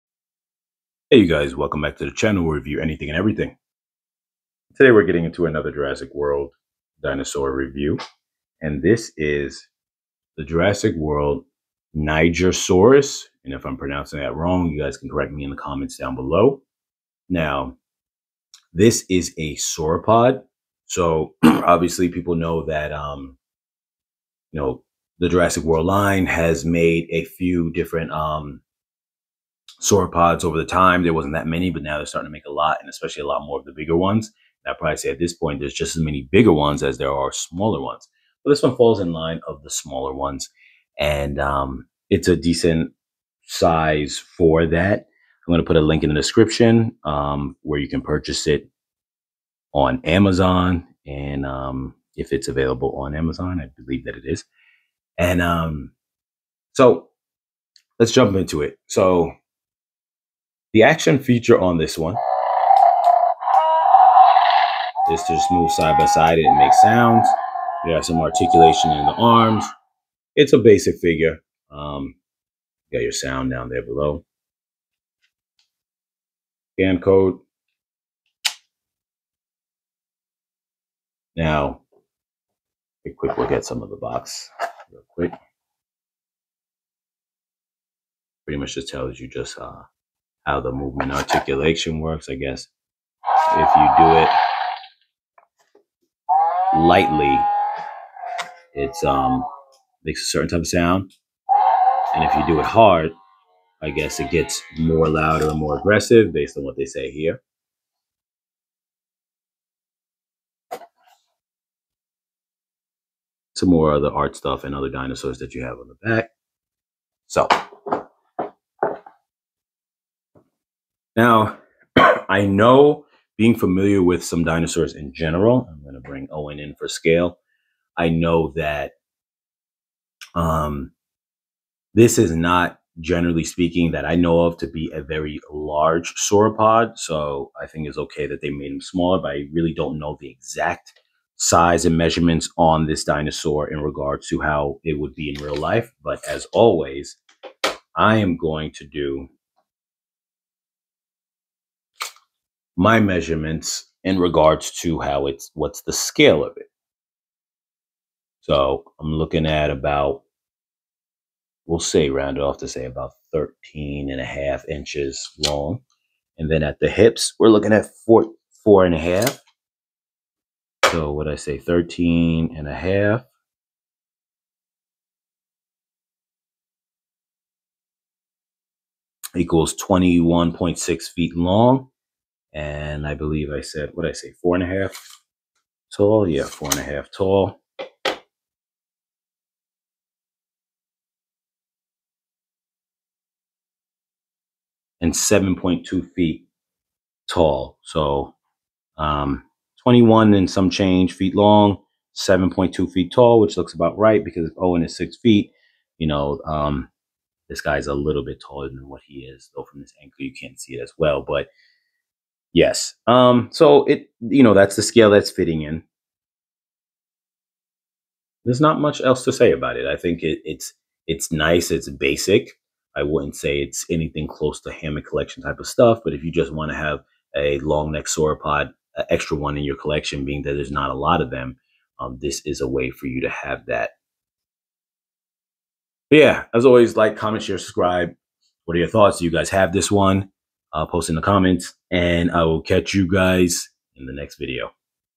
Mattel Jurassic World Dino Trackers Nigersaurus Review
Mattel Jurassic World Wild Roar Dinosaur Toy with Sound & Attack Move, Nigersaurus Posable Action Figure, 11 inches Long